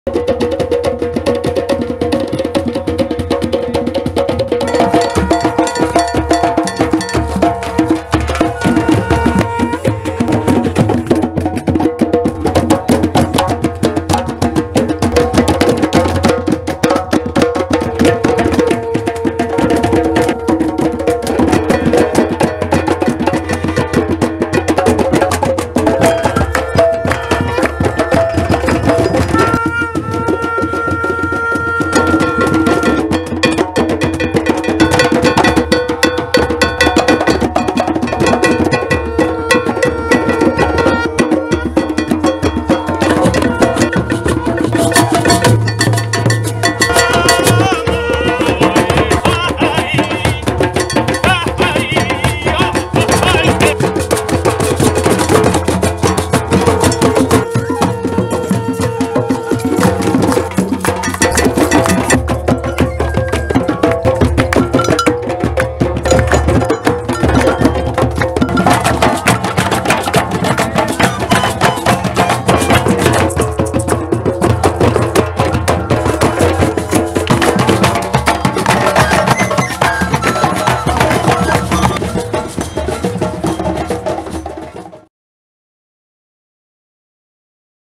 Drum Circle - Miraflores, Lima, Peru
Drummers and spectators meet Sunday afternoons in Maria Reiche Park in Miraflores.